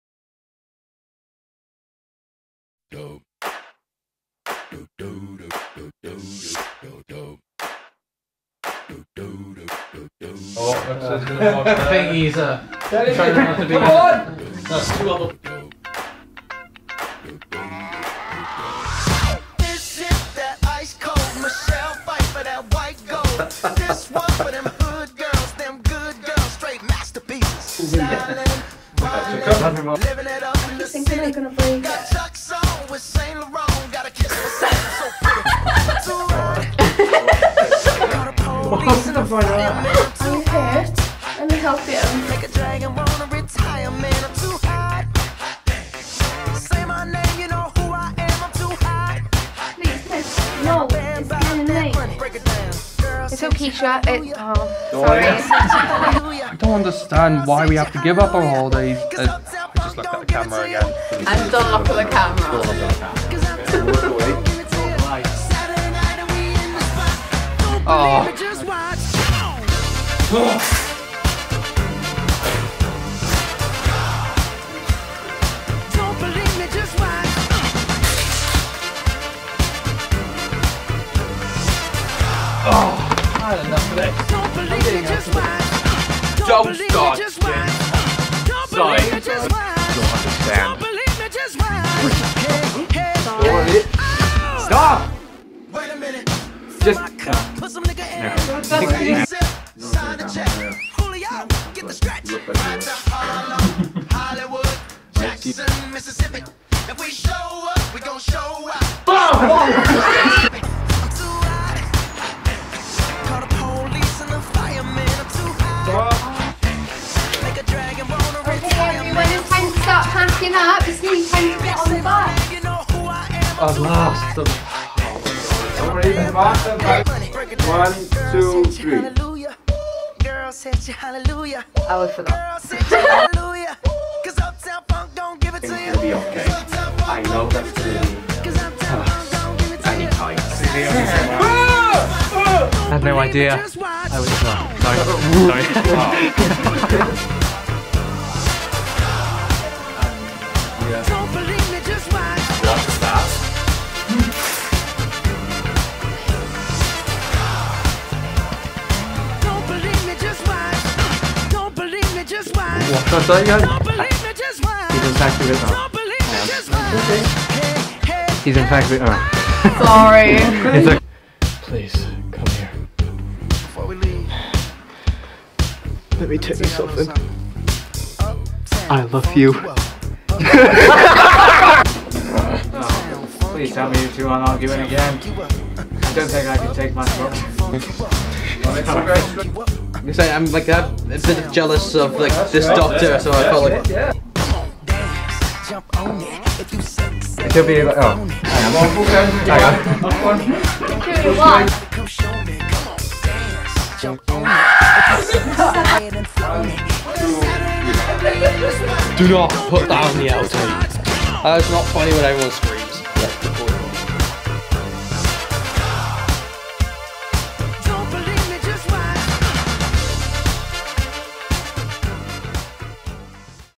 DOCTOR WHO BLOOPERS & OUTTAKES - AUTUMN 2023
We have had lots of fun making this terms Doctor Who Film Skills project, take a look at some of our funniest bloopers and outtakes.